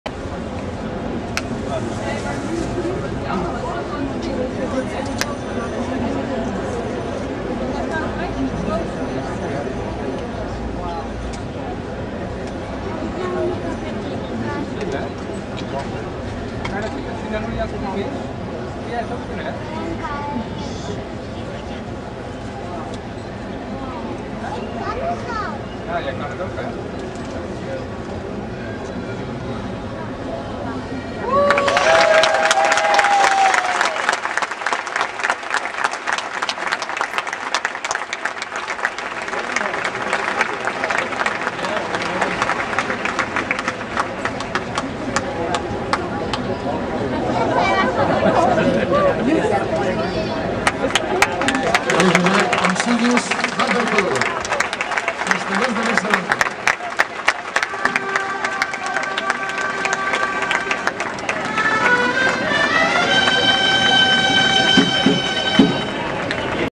Castellers de Barcelona: pd5 de dol Aniversari colles històriques 11/07/2009
Pilar de cinc de dol dels Castellers de Barcelona l'11 de juliol del 2009 a la Plaça de Sant Jaume de Barcelona amb motiu de la diada d'aniversari de colles històriques.